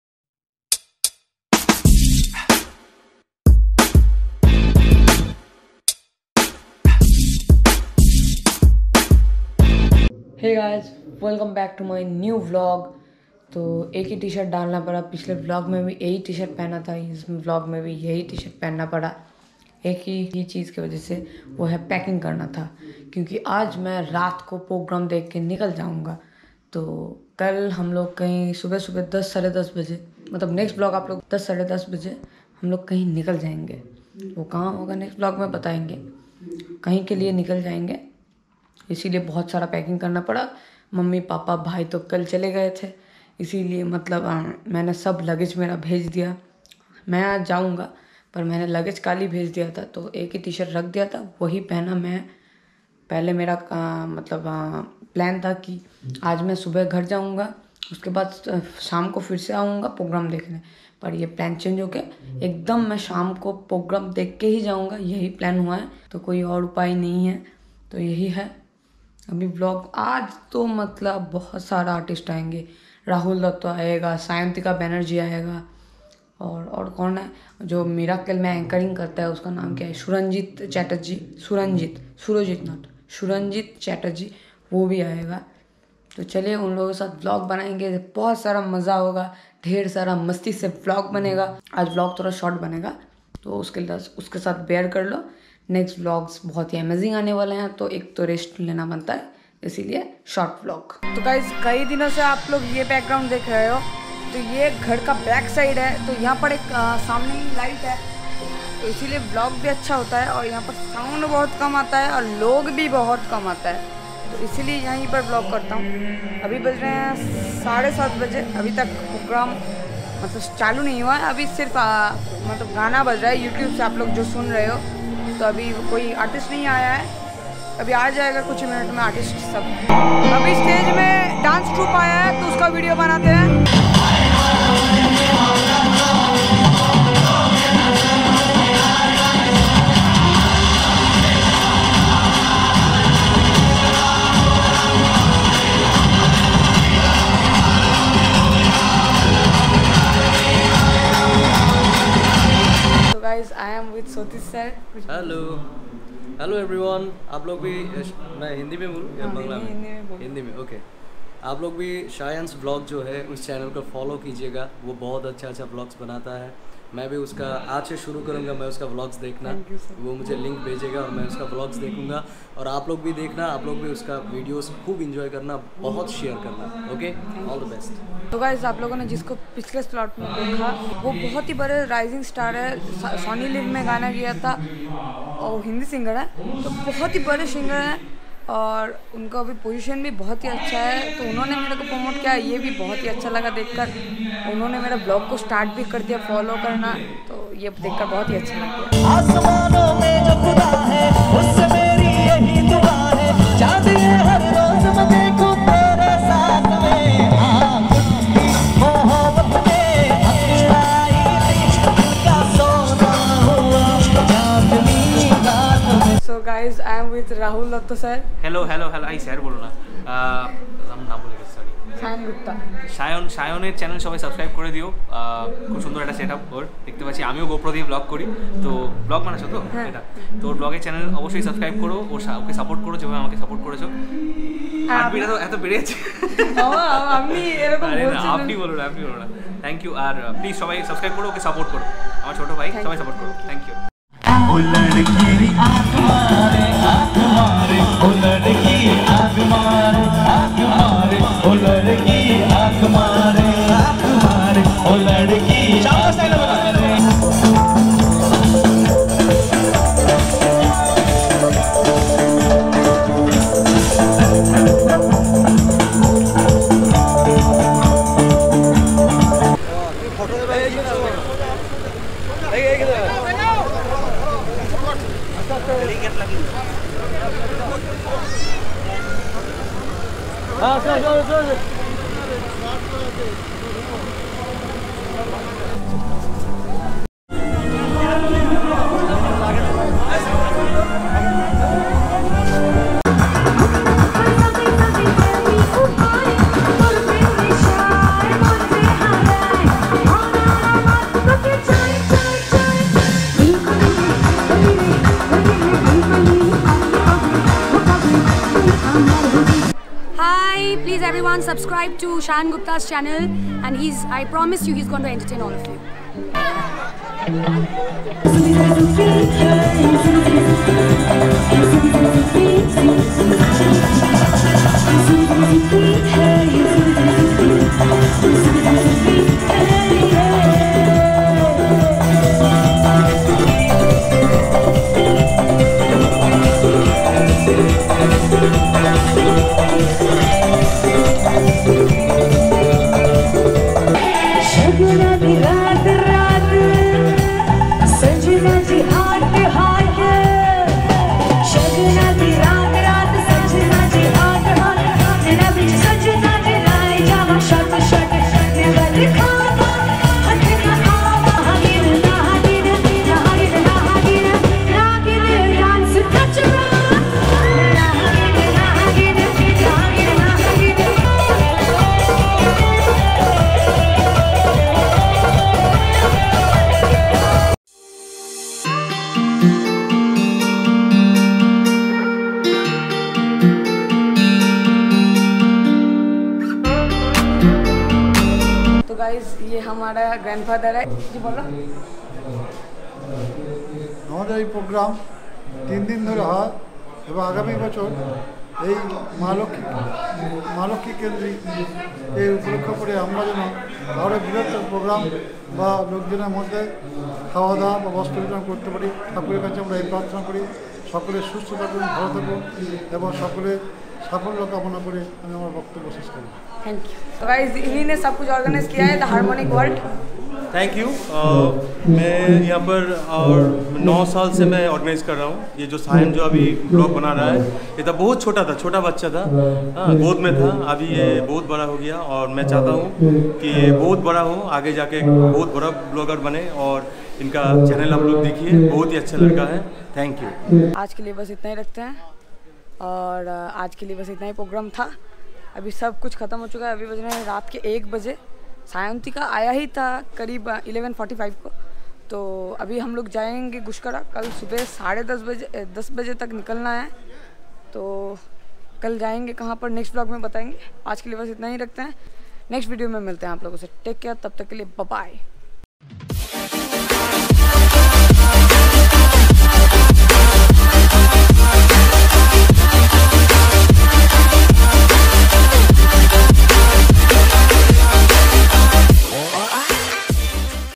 SAYANTIKA BANERJEE & @RahulOfficial IN VLOG || SPECIAL VIDEO || LAXMI PUJA FUNCTION || VLOG-45
IN THIS VIDEO I MET SAYANTIKA BANERJEE & RAHUL DUTTA . ENJOYED A LOT IN THE LAST DAY OF LAXMI PUJA FUNCTION .